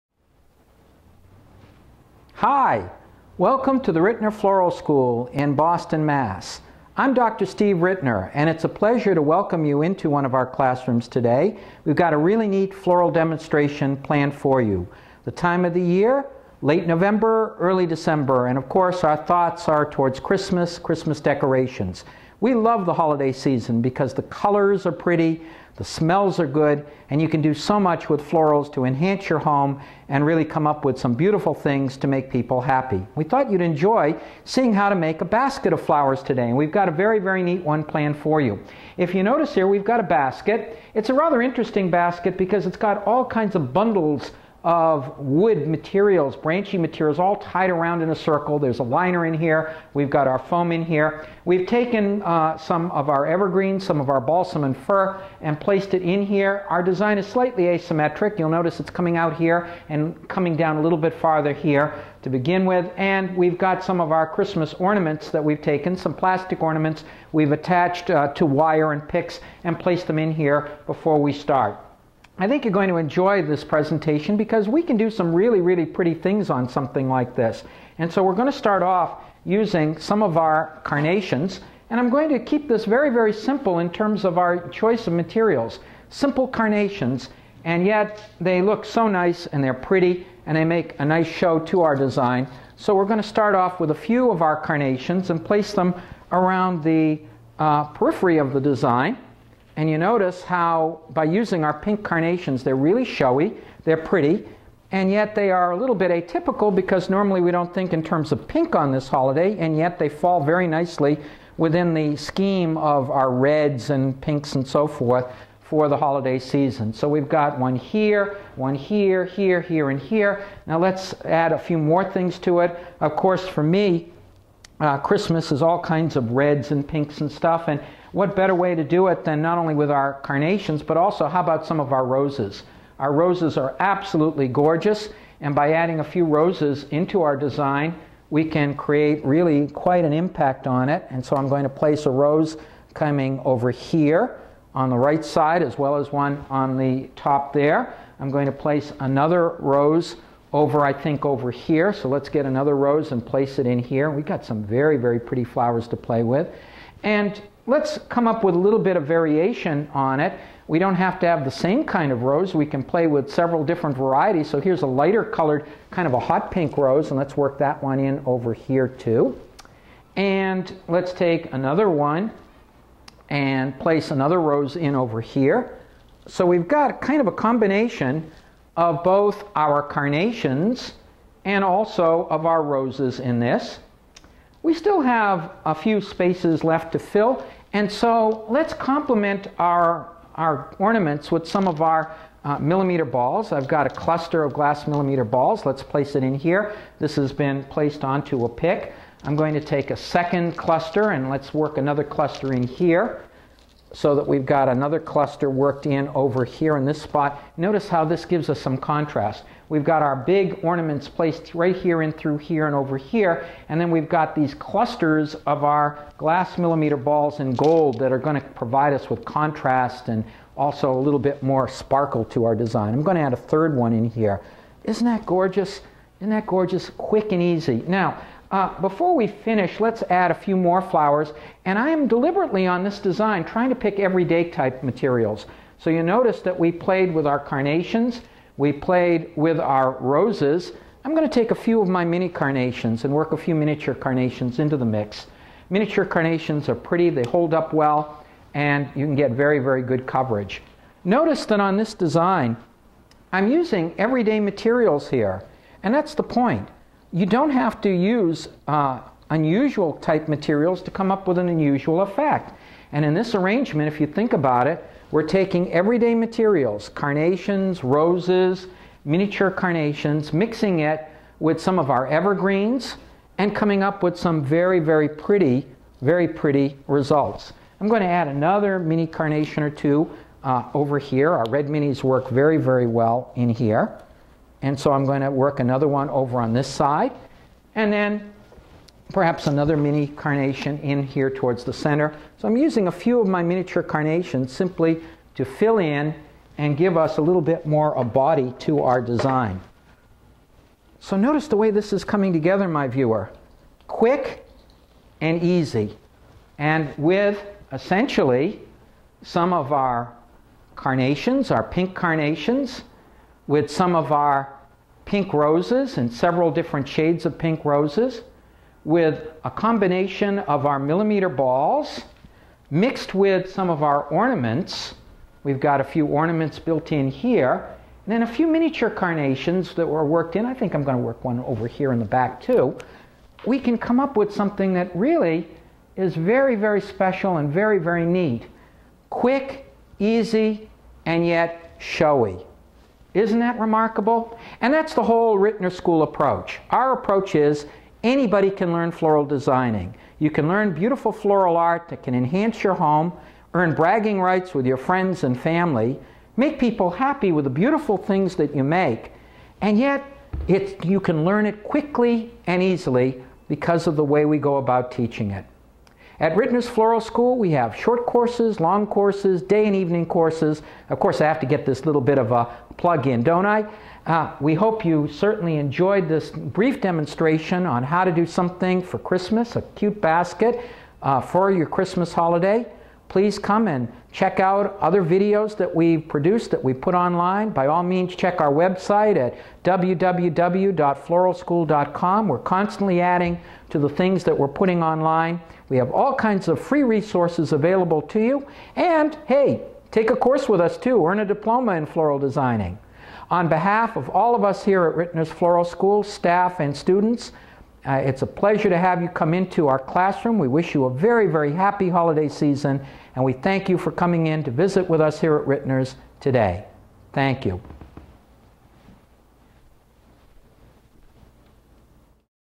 Great Christmas Floral Basket
At Rittners Floral School, in Boston, we definitely enjoy the Christmas season. In this video we show you how to create a beautiful basket of flower using seasonal colors and materials...!

Quick! Easy! And magnificent!

Enjoy! And from all of us at Rittners Floral School,  have a lovely holiday season.